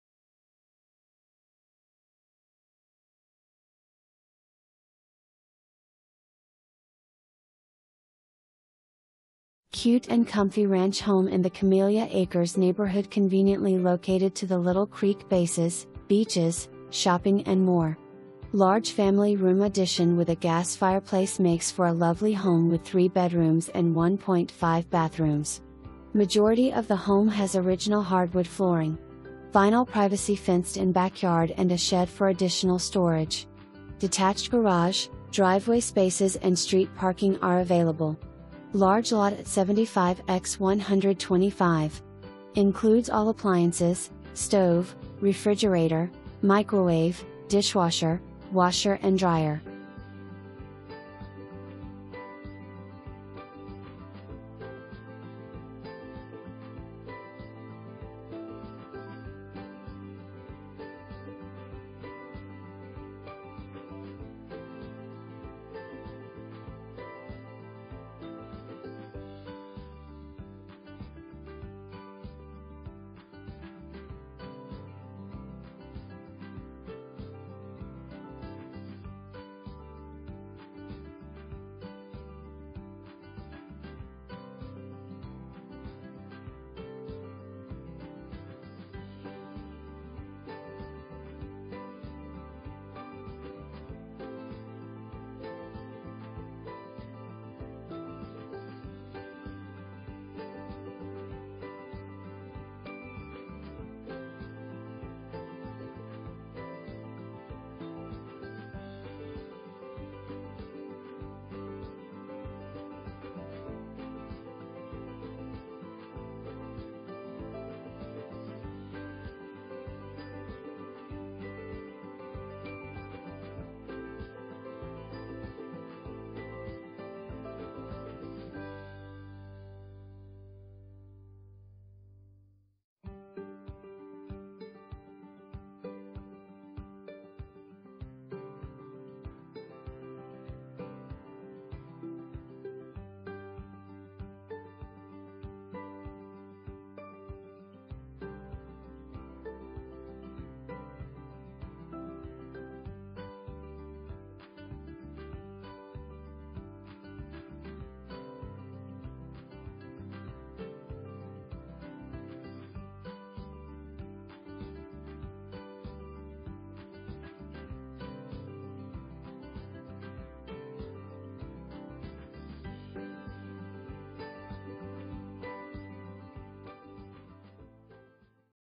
2528 E Little Creek Road
2528 E Little Creek Road

Cute & Comfy Ranch Home in the Camellia Acres Neighborhood conveniently located to the Little Creek Bases, Beaches, Shopping and more. Large Family room addition with a Gas Fireplace makes for a lovely home with 3 Bedrooms and 1.5 Bathrooms. Majority of the Home has Original Hardwood Flooring. Vinyl Privacy Fenced in Backyard and a Shed for additional Storage. Detached Garage, Driveway spaces and Street Parking are available. Large Lot at 75' x 125'. Includes all appliances (Stove, Refrigerator, Microwave, Dishwasher, Washer & Dryer).

1 full bath, 1 half bath

Wendy Plemmons
Rose & Womble Realty Company